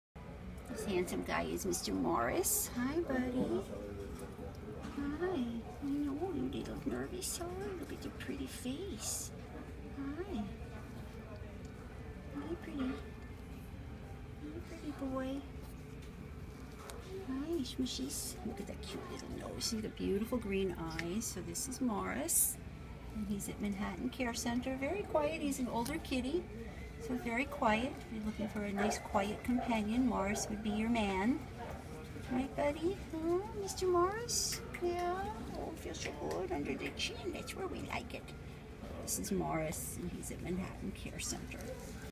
Morris A1093681 SAFE
My name is MORRIS. My Animal ID # is A1093681.
I am a neutered male black and white domestic sh mix. The shelter thinks I am about 17 YEARS old.

I came in the shelter as a STRAY on 10/16/2016 from NY 11365, owner surrender reason stated was STRAY. I came in with Group/Litter K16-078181.

MOST RECENT MEDICAL INFORMATION AND WEIGHT
10/21/2016 Exam Type RE-EXAM - Medical Rating is 4 NC - SEVERE CONDITIONS NOT CONTAGIOUS, Behavior Rating is NONE, Weight 12.5 LBS.

10/21/16 14:18 S: much nicer than on intake exam, allowed handling well O: - CV: 3/6 left sidfed heart murmur, strong synchronous pulses, pink moist muc membranes CRT 2sec - Resp: WNL - MS: BCS 9/9 - Neuro: no obvious deficits - Abd: soft non painful, no masses or organomegaly A: - heart dz vs other - obese P: recommend echo px open based on results of further testing

10/16/2016 PET PROFILE MEMO
10/16/16 18:13 Morris allowed staff to collar, scan for amcirochip and take pictures with no signs of aggression

10/19/2016 BEHAVIOR EVALUATION - AVERAGE
Exam Type BEHAVIOR
Reaction to assessor: Morris looks neutral, crouched position in his litter box when appproached by the assessor. Reaction to door opening: Morris remains in place, alert with soft eyes. Reaction to touch: Morris leans into the assessor's hand and appreciates petting on the head and body. Reaction to Being Picked up: He becomes antsy then jumps back into the cage. Placement determination: Average Morris interacts with the observer, appreciates attention, is easy to handle and tolerates all petting. This cat is showing behavior appropriate for new or experienced cat parents.

GROUP BEHAVIOR EVALUATION
No Group Behavior Summary

10/16/2016 INITIAL PHYSICAL EXAM
Medical rating was 3 NC - MAJOR CONDITIONS NOT CONTAGIOUS, behavior rating was NONE
scanned negative neutered male ou clear au clean no nasal discharge teeth:possible dental disease ,placed on vet check nosf ambx4 haircoat:dirty flea comb negative injuries:none seen during exam declawed:front paws declawed rear paw nail intact behavior:slight nervous but allowed handling nosf

Brooklyn Center